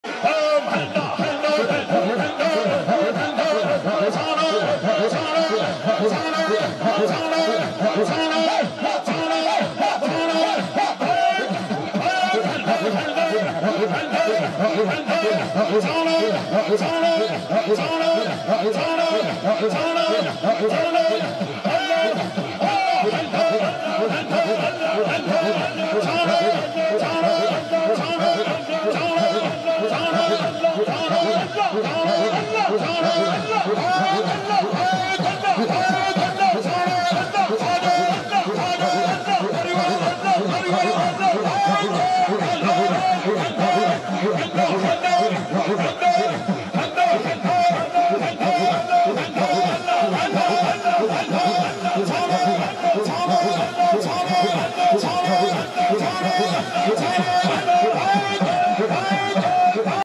Zikr Allah Hu - Wajd - Subhan Allah
ذکر اللہ ھو ۔ وجد ۔ سبحان اللہ
دربار عالیہ اللہ ھو والا ڈیرہ گلستان کالونی نمبر 1 فیصل آباد پنجاب پاکستان